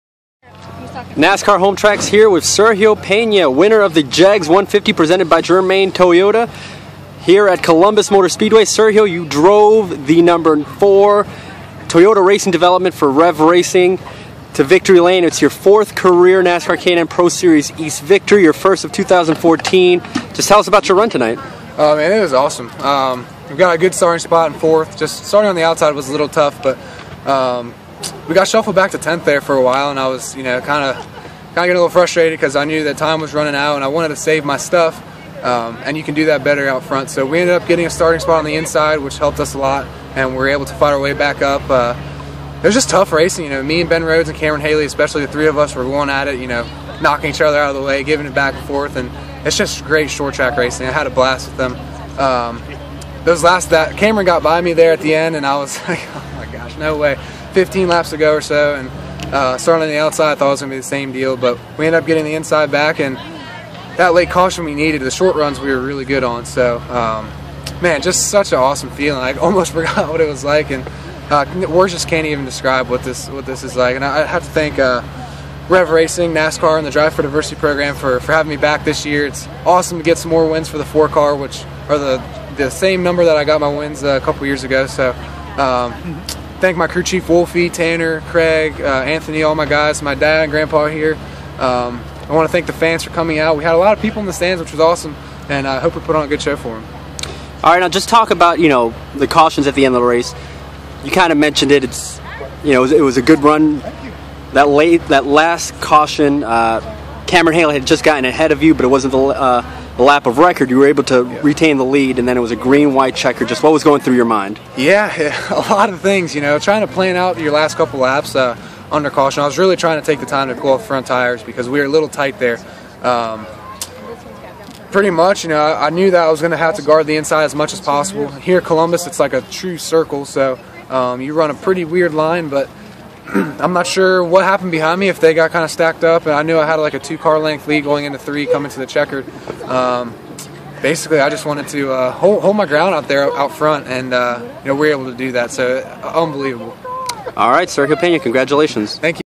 2014 NKNPSE Columbus Sergio Pena Victory Lane
Sergio Pena, driver of the No. 4 Toyota Racing Development Toyota for Rev Racing, wins his fourth career NASCAR K&N Pro Series East race in the JEGS 150 presented by Germain Toyota at Columbus Motor Speedway.